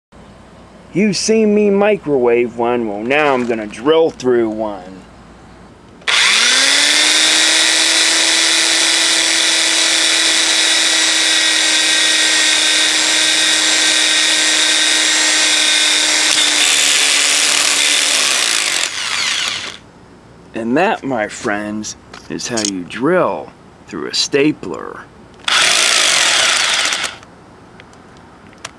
Drilling Into A Stapler
Episode 41: Stapler

(Rated G) It wasn't that long ago that I microwaved a stapler, so why not drill through one too? I know in this video I only drill part of the way into the stapler, but the other part wouldn't secure to drill into, when I tried to drill through the other part the stapler bounced out and shot like a rocket, I wish I would've recorded that but sadly I forgot to hit "record" (no idea why that's such an amateur mistake that I thought I was beyond doing). Either way this wasn't quite as interesting as the microwave video but was definitely quicker. It's also getting near the end of "Drill This" season, only a few more to go before the season finale in October. Still not sure what it'll be yet but I have some ideas.